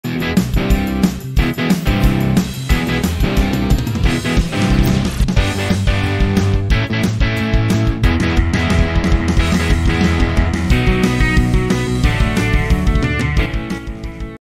How to set the PAS mode on display
1, If  you disassemble the thumb throttle, need to set it to 000(pure PAS mode)
2, If you assemble the thumb throttle, need to set it to 002 (hybrid mode)